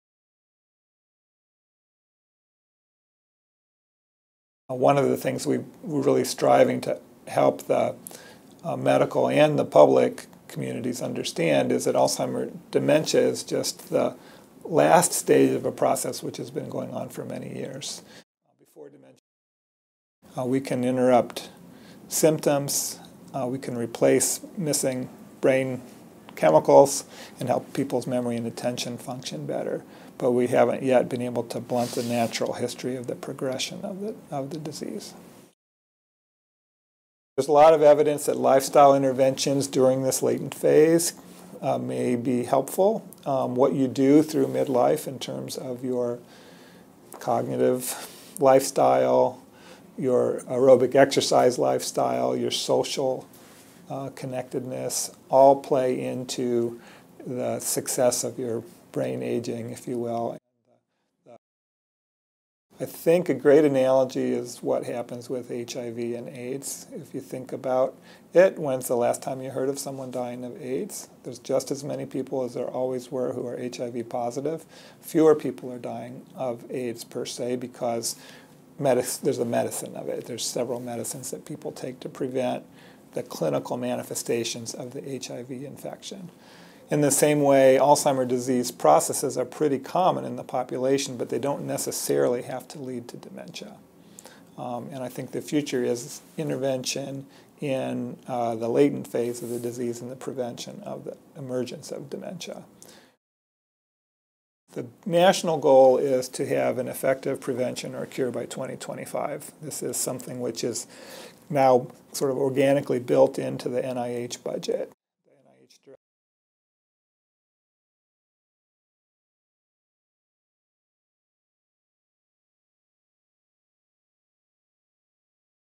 Alzheimer's disease and dementia
Former Supreme Court Justice Sandra Day O'Connor has been diagnosed with dementia and likely Alzheimer’s disease. Hear from Dr. Thomas Grabowski, director of the UW Medicine Brain and Wellness Center in Seattle. He talks about the current understanding of Alzheimer’s disease and how it appears in people.